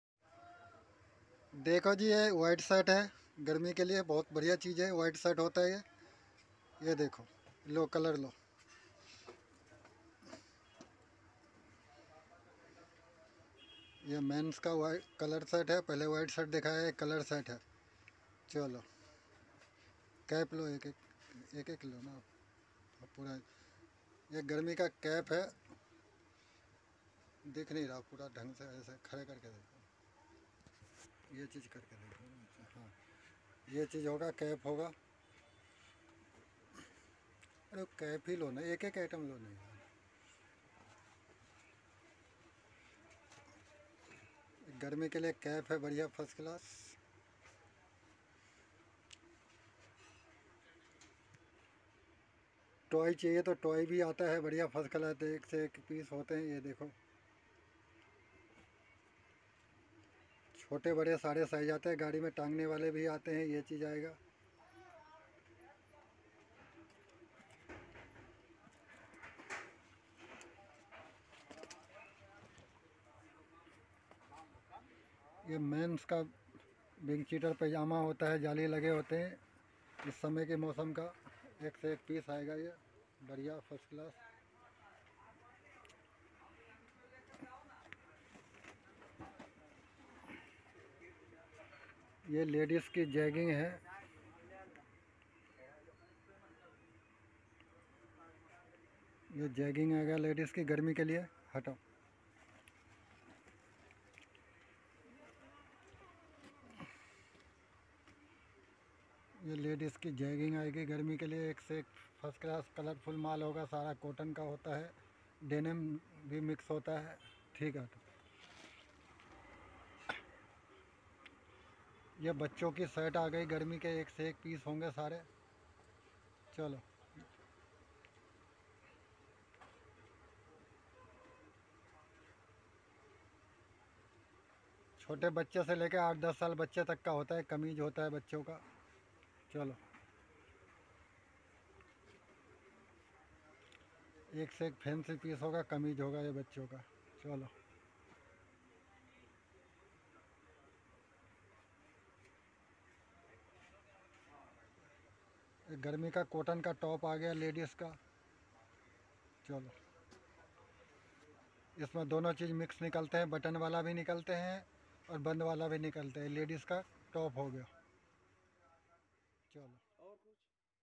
कैफ टोयल || Shirt T-Shart All Items Available | Azad Market Delhi In Wholesale | Old Cloth
कैफ टोयल || Shart T-Shart All  Items Available | Azad Market Delhi In Wholesale | Old Cloth

Hello Friends Aaj Is Video Me Aapko Sarpuls kapda Dikhaya hai Aap Log End Tak Video Dekhe Fir Oder Kare Thanks 👍 So Much

Address--- Azad Market Shivaji Rod Shop No 20

My New boya m1 mic